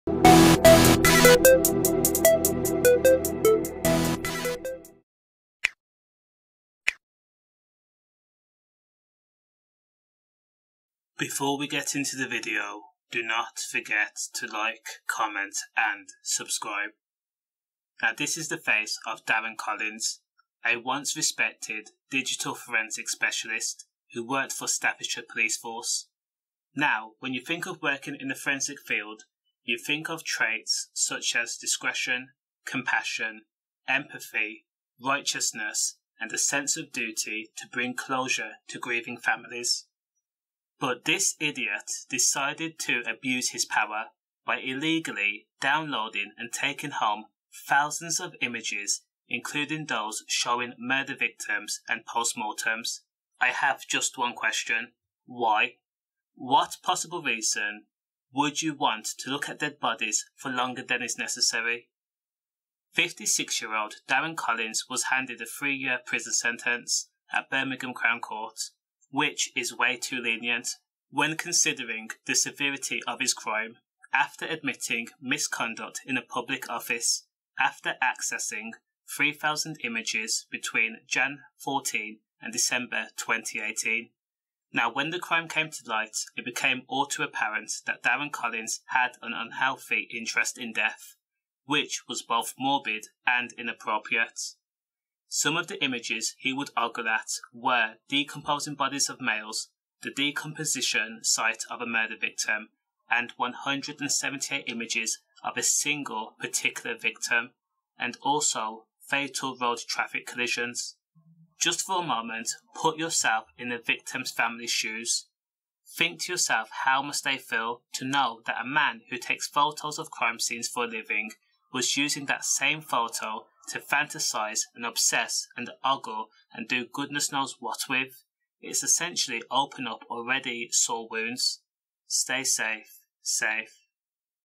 Police Worker Darren Collins Jailed For Taking Home Pictures Of Murder Victims
This is the face of a former digital forensic specialist who has been locked up today for illegally downloading and taking home thousands of images including those showing murder victims and post mortems.

Darren Collins, aged 56, was handed a three year prison sentence after admitting misconduct in a public office after accessing over 3,000 images between January 2014 and December 2018 while working for Staffordshire Police.

Safe